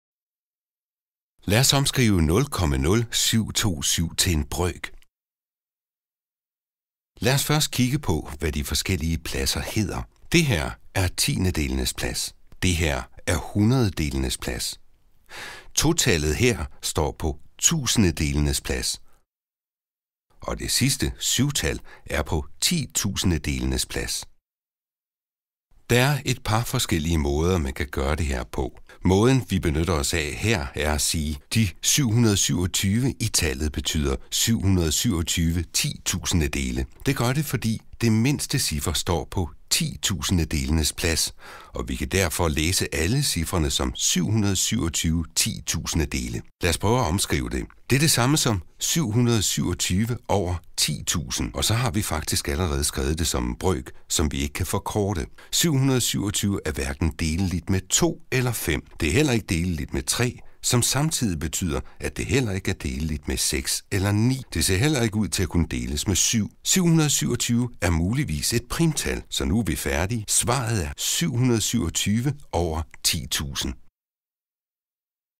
Omskrivning af decimaltal til brøker 2 (eks. 1)
Original titel: "Converting decimals to fractions 2 (ex 1)"

Vi omskriver et decimaltal til en brøk ved at kigge på cifrenes placering, i det her tilfælde tiendedelens, hundrededelenes, tusindedelenes og ti-tusindedelenes plads.